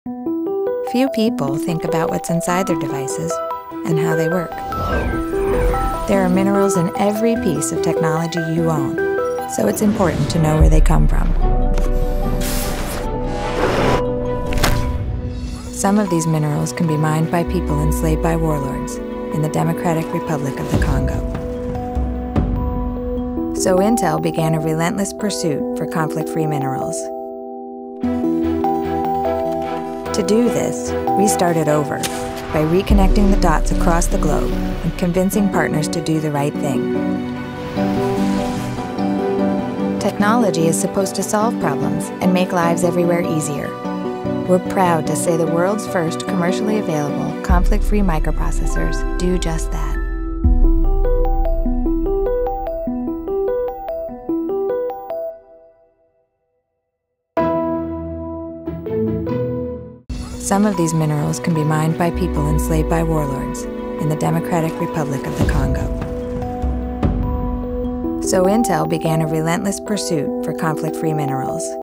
Look Inside.™: In Pursuit Of Conflict-Free | Intel
Intel's quest to eliminate from our supply chain conflict minerals that fund violence gives people a 'conflict-free' choice. By reconnecting the dots across the globe, Intel proudly manufactures processors that are DRC conflict-free.